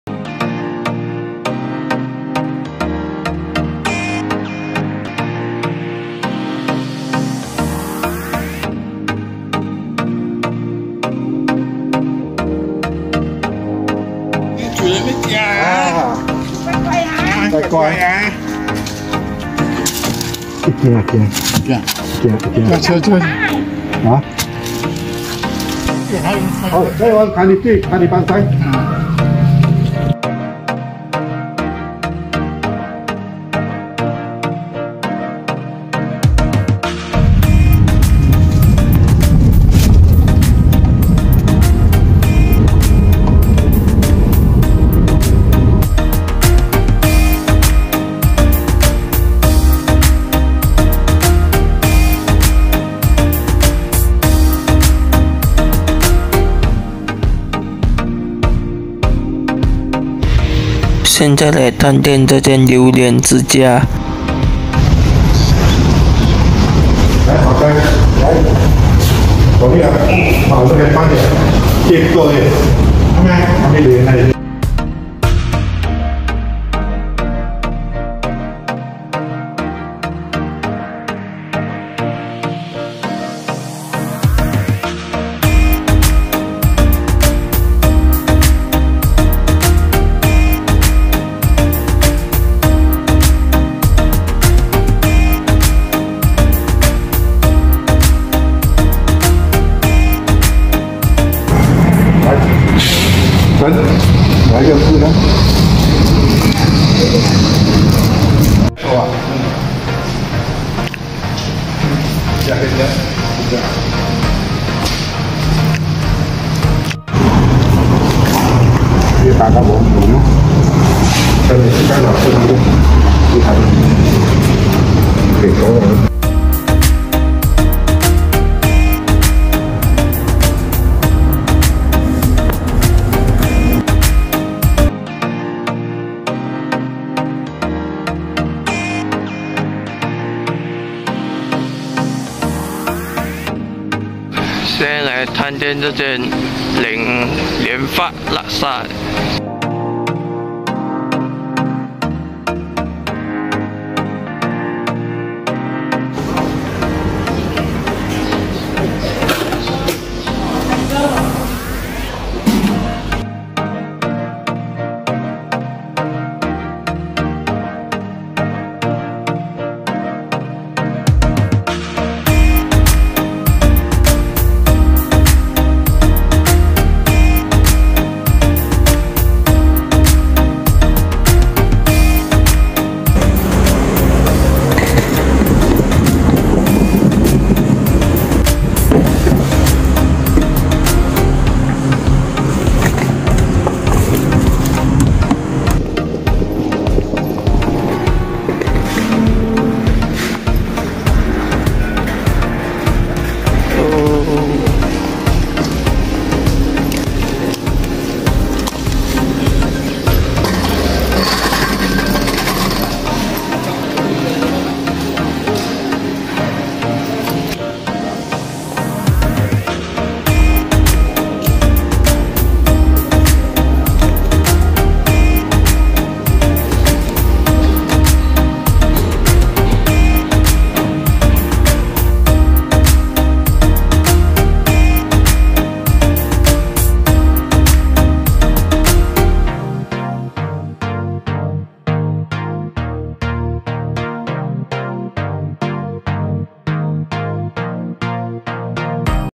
美食篇【Vlog270】 ： 被感染后的吉打 第34集 之 吃遍吉打 EP 9
探店/美食 :
~打包四大天王的午餐
~榴莲之家
~连发Laksa
~ALor Star Aiport